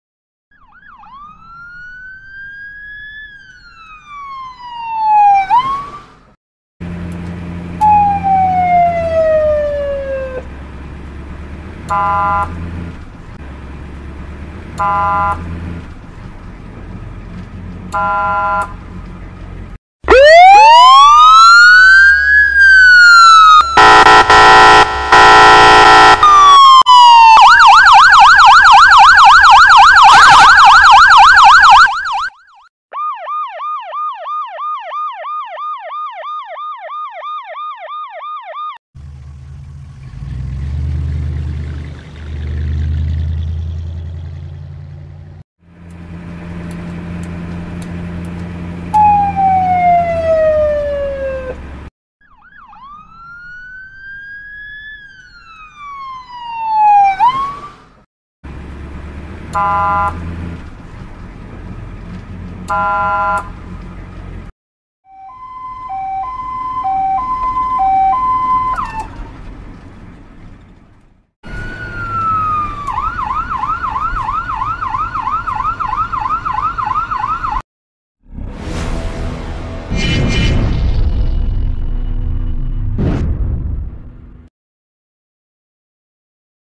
AMBULANCE VIDEO ON THE GREEN SCREEN. FOOTAGE ANIMATION.
Footage of an ambulance on a green screen for video editing. Video animation of an ambulance from different angles to insert into the video. Use it in your videos.

CHANNEL ARMY